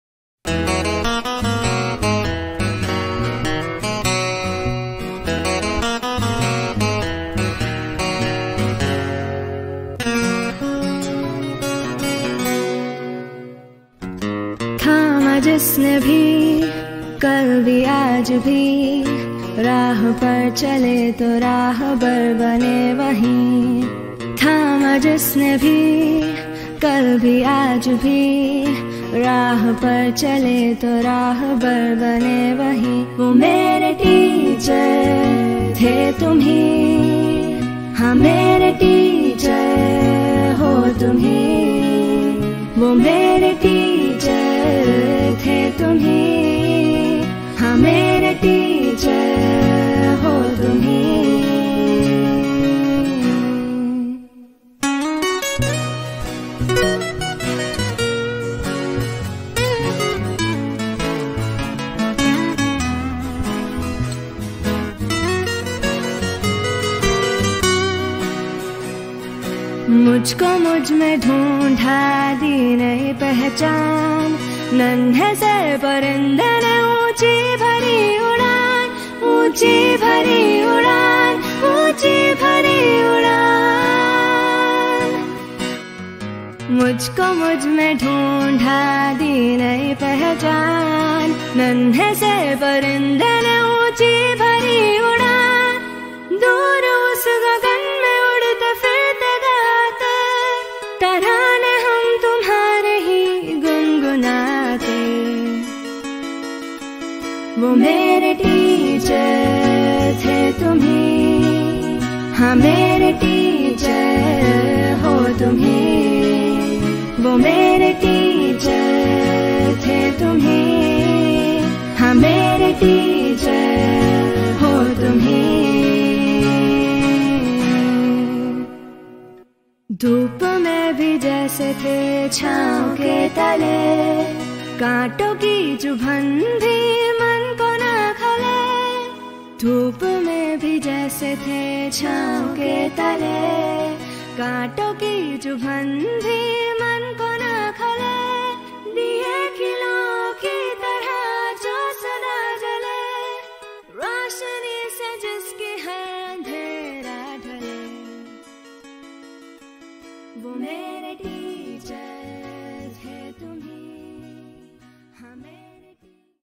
🔴TEACHER'S DAY CELEBRATION || K.C.Y.M PUDUKAD || TRICHUR ARCHDIOCESE
Honouring dear teachers from Pudukad parish by giving them a sapling.
An Initiative of K.C.Y.M. Pudukad.

Hats off dear teachers for your valuable and selfless services .Heartfelt gratitude to you for moulding us.